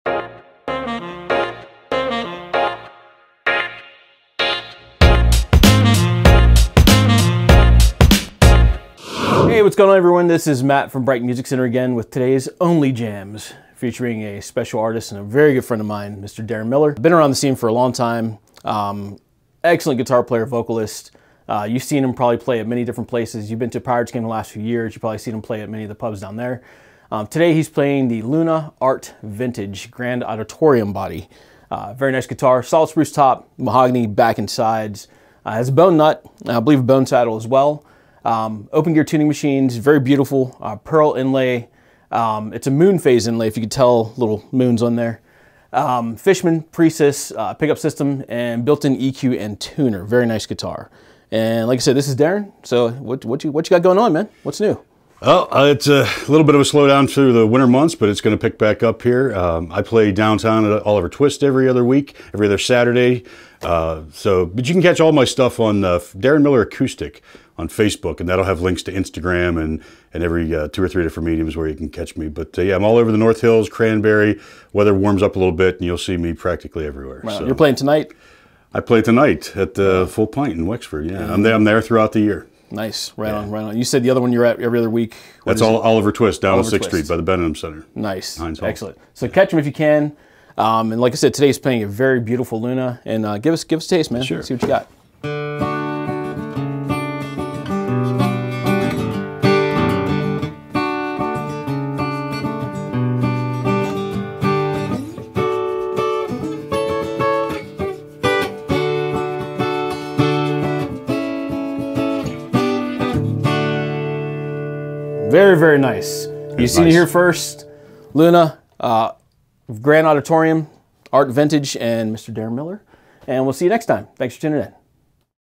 Darren Miller | Luna Art Vintage Grand Auditorium CAW A/E
When we speak of tone, we expect great response and clarity. Look no further!! The Luna Art Vintage Grand Auditorium delivers!!! With a solid spruce top and mahogany back and sides, rich, dynamic sounds are yours to shape and create! This lunar guitar allows you to transfer your method of Expression straight to your fingertips without hesitation!
Stop by both locations to pick yours up today!!